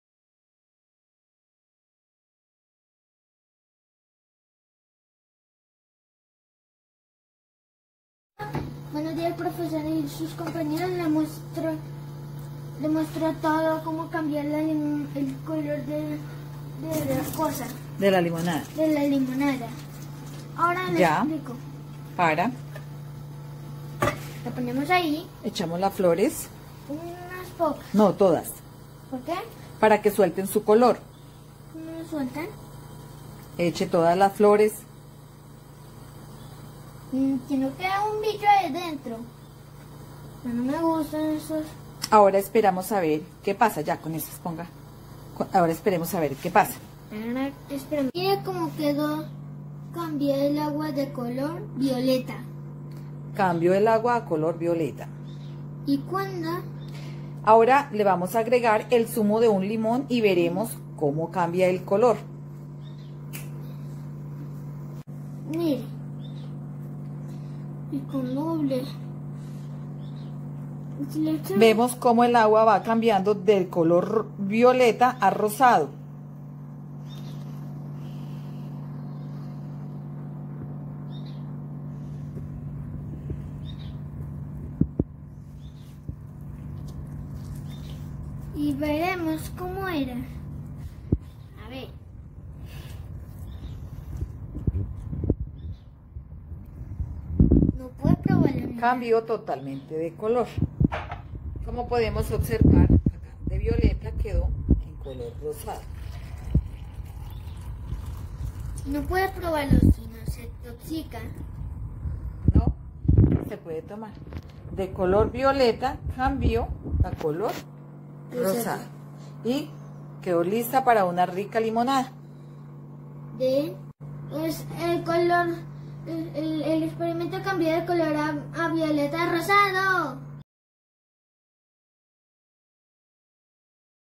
Como cambiar el color de una limonada. Presentado por Maximiliano Morales. 3°
Actividad institucional.

DIA DE LA CIENCIA LIMAC.

Grado tercero.

2.020